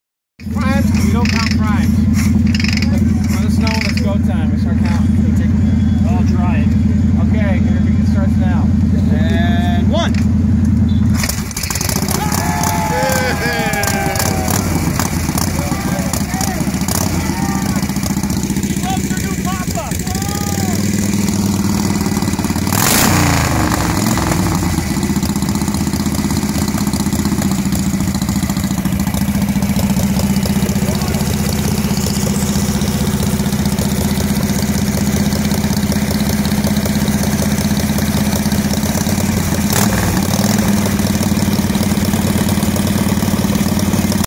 Old School Kickstart at Murdercycles' Bike Night
Murdercycles Bike Night, May 11, 2016 - Reid's Ice Cream, Coventry CT